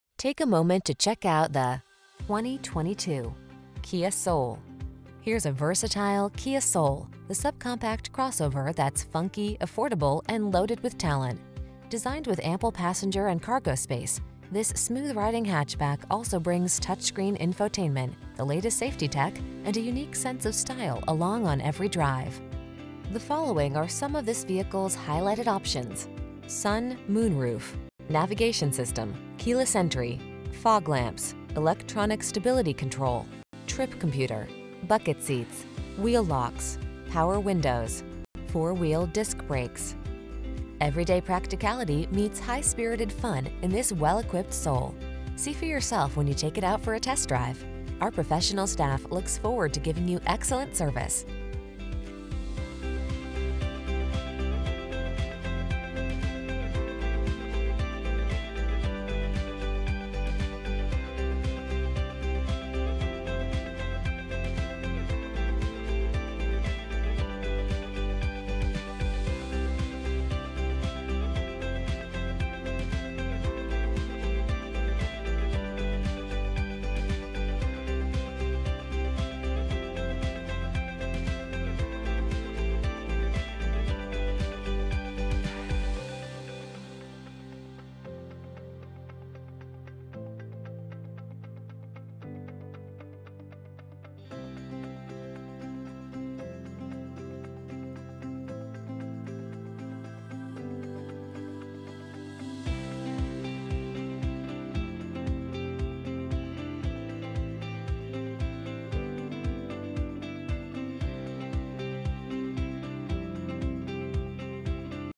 2022 Kia Soul Arlington Heights, Buffalo Grove, Des Plaines, Skokie, Glenview, IL K14561
Inferno Red New 2022 Kia Soul available in Arlington Heights, Illinois at McGrath Arlington Kia. Servicing the Buffalo Grove, Des Plaines, Skokie, Glenview, IL area

1400 E. Dundee rd

2022 Kia Soul GT-Line Inferno Red Soul FWD IVT 2.0L I4 DOHCbrbr28/33 City/Highway MPG